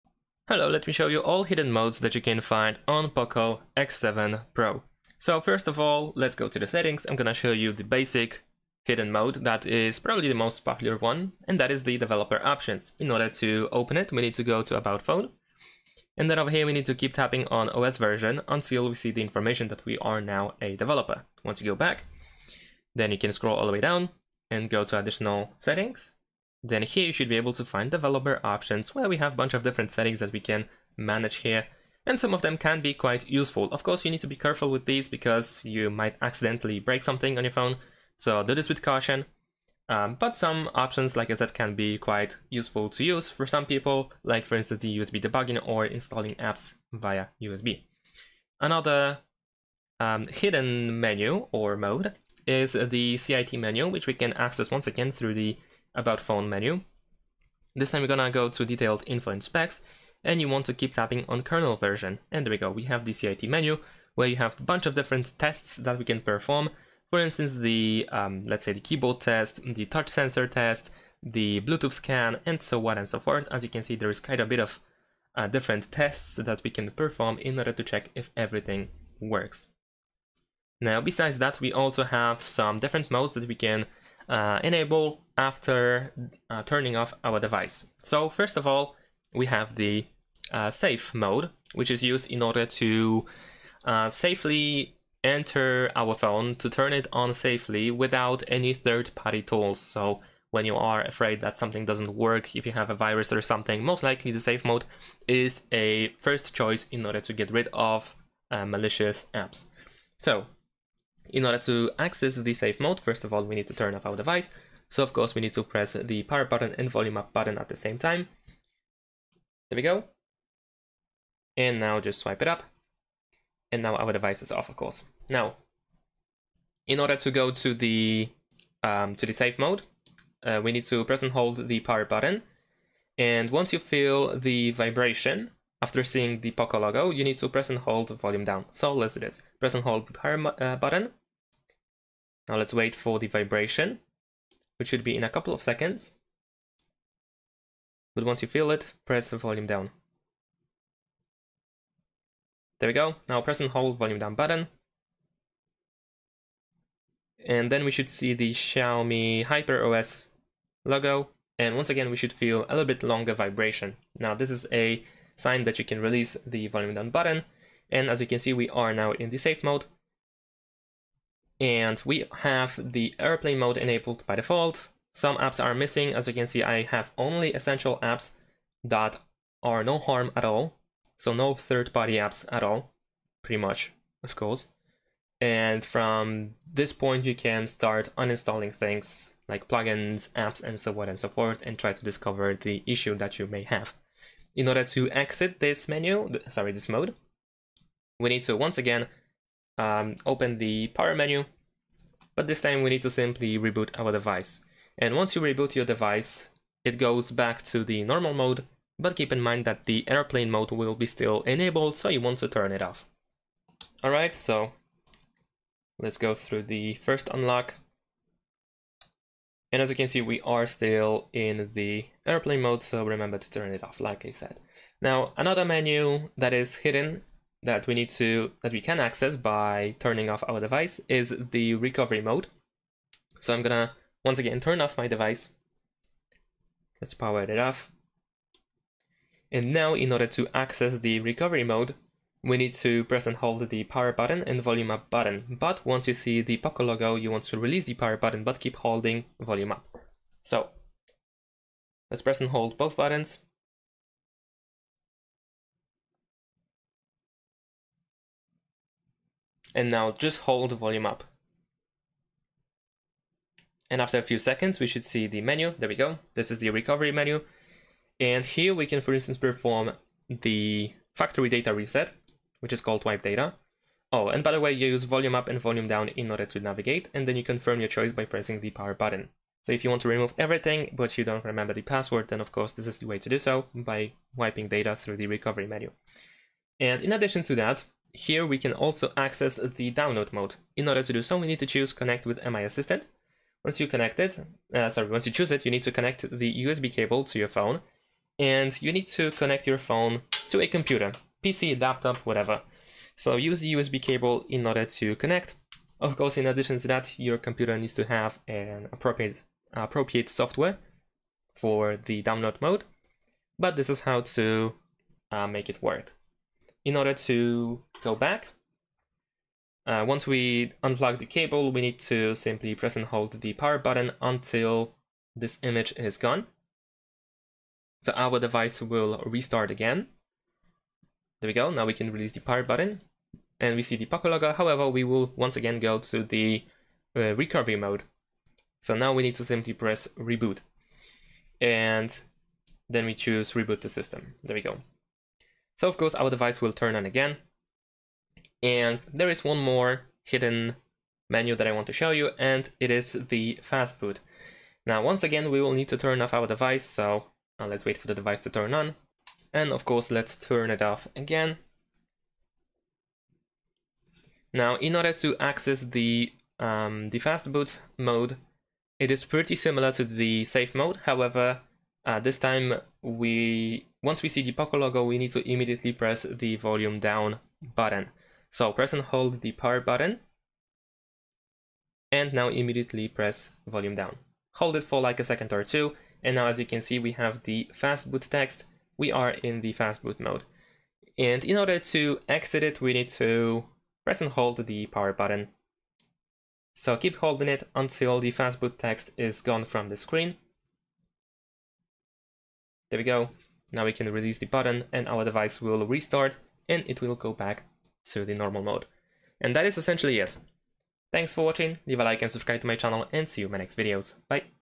POCO X7 Pro: All Hidden Modes
Discover all the hidden modes available on the POCO X7 Pro in this comprehensive guide. Learn how to access advanced features like Fastboot Mode, Recovery Mode, Download Mode, and more to troubleshoot, optimize, and explore your device. Perfect for unlocking its full potential and mastering every functionality. Watch now to uncover the hidden modes of your POCO X7 Pro!